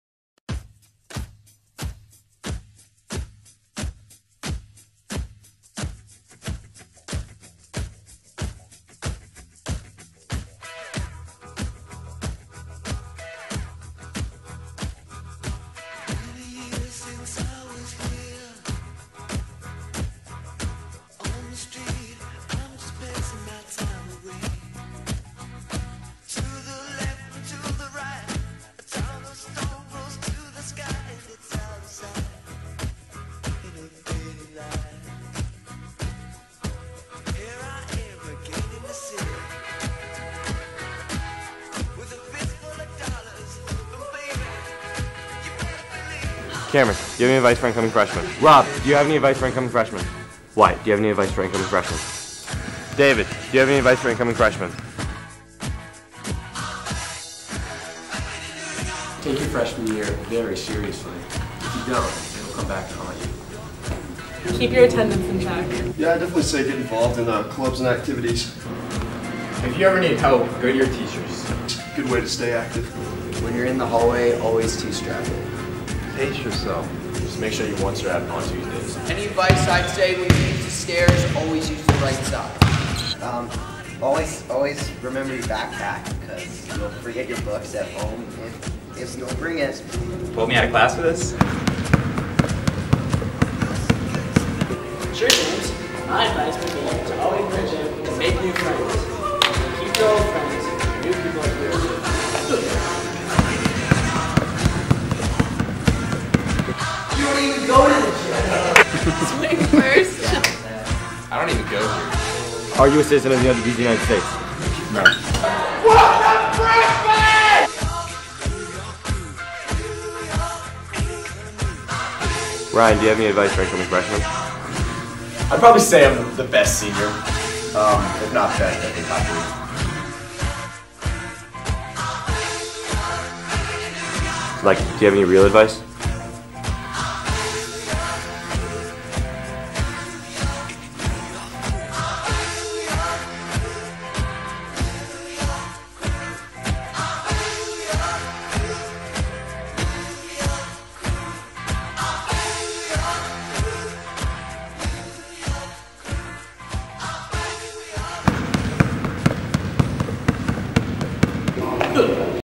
Welcome Freshman
Welcome Yorktown High Schools Class of 2018 over and over again in this what-will-happen-next?, sit-com of a promo.